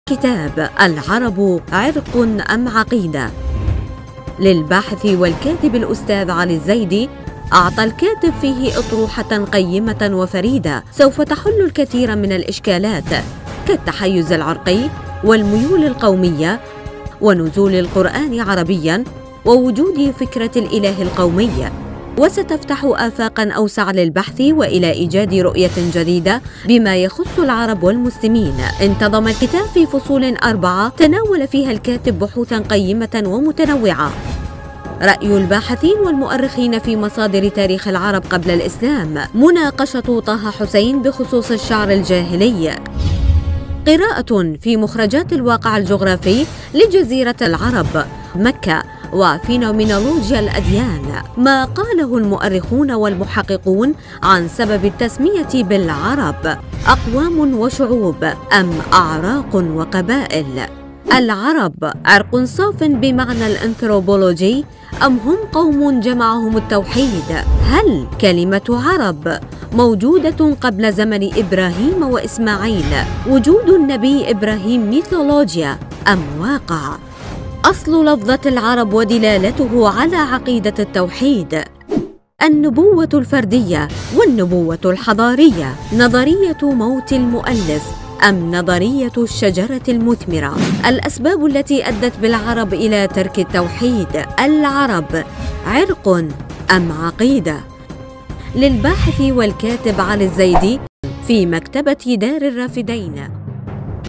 كتاب "العرب عِرق أم عقيدة؟" للباحث والكاتب الأستاذ علي الزيدي
من  الأمور التي أسست لها مفهوماً في الذهنية العربية أو الشعوب الأخرى، هو التصور المطروح عن أصل العرب، ومن أين جاءتهم هذه التسمية، بحيث تلبست بهم، هذا التلبس الذي لا يمكن أن يتصور بأن يوماً ما سيزال عنهم، فأصبح وكأنّه ملازماً ذاتياً لهم.
وقد ذكر المؤرخون والباحثون عن أصل كلمة (عرب)، وعن أصل المادة التي اشتقت منها الكلمة، وما هو أقدم ذكر لها، آراءً كثيرة، فما كان من أكثرها، إلّا أن يكـون حدسـاً أو وهمـاً، حتى وصل الأمــر بأحدهم أن يذكر ثلاثة عشر وجهاً من وجوه الإشتقاق للفظ العرب، وكان كثير من الوجوه التي ذكرها، بما فيها من تكلف، فإنها تنطبق بشكل أو بآخر على غير الشعوب العربية أيضاً.
وهم قد تناسوا أو غفلوا عن أهمية هذا الموضوع، حينما تناولوه بالبحث والتدقيق، فصاغوا عنه آراءً هي أقرب للسطحية من التحقيق العلمي المنتج. وذلك لما سيثار حوله من إشكالات وأفكار نقدية، قد تولد كثيراً من الشكوك والتساؤلات باعتبار التحيز العِر-قي والميول القومية ونزول القرآن عربياً، ووجود فكرة الإله القومي، الى غيرها من الإعتبارات المرتبطة بالموضوع، خصوصاً إذا ما نظر إليه بتحسس بعض الشيء، أو حتى لإيجاد فرصة لنقد الإسلام من الداخل أو الخارج.
وهذا ما سنبحثه ونبيّنه في طيّات كتابنا هذا. فكان لزاماً علينا ترك ما هو مألوف وشائع بصيغته الحالية، والبحث عن مخرجات جديدة، تتبنى قضية مهمة وهي الكشف عن أصل كلمة (عرب) لنجده بالتالي بعيد كل البعد عمّا هو متداول بيننا.
احصل على نسختك من مقر مؤسسة جامع الائمة للثقافة والإعلام